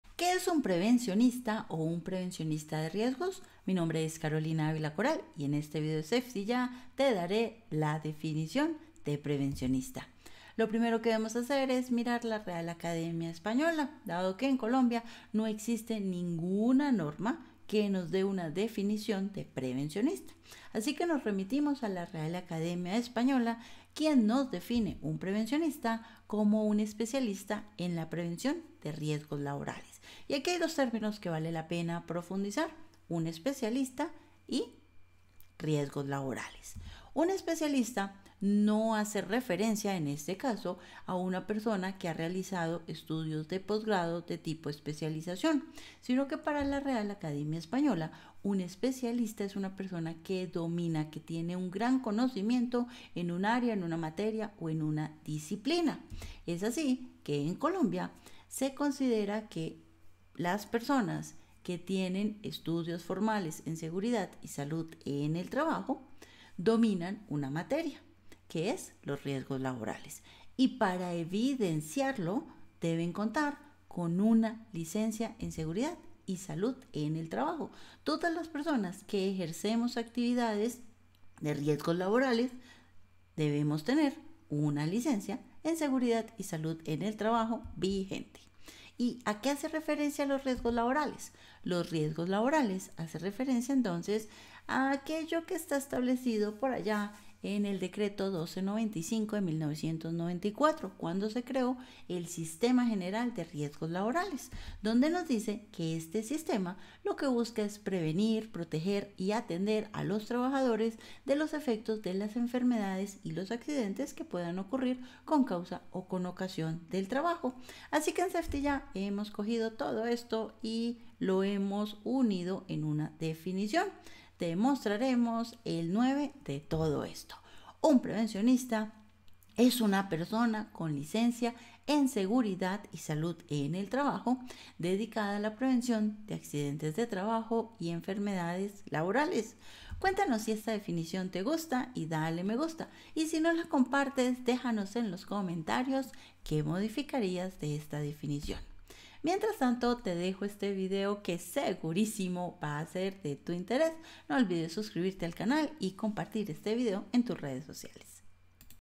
Qué es un PREVENCIONISTA 📗 [Definición]
Se habla de prevencionista o prevencionista de riesgos, te contamos la definición según la RAE y nuestra adaptación a Colombia.
👇👇 ver MÁS info 👇👇

Prevencionista es un término que se escucha en las empresas, pero muchas veces no conocemos exactamente qué significa, es por eso que en SafetYA hemos realizado este video analizado varios aspectos de su definición.

0:00 ¿Qué es un prevencionista de riesgos?
0:10 Prevencionista según la RAE
0:36 ¿Qué es un especialista?
1:01 Estudios formales en seguridad y salud en el trabajo
1:19 Licencia en seguridad y salud en el trabajo
1:30 Riesgos laborales
1:58 Qué es un prevencionista en Colombia

**Artículos relacionados **
⭐ Definición según la RAE

** Nuestra revista virtual **
SafetYA es una revista virtual de seguridad y salud en el trabajo, todas nuestras publicaciones las puedes encontrar en: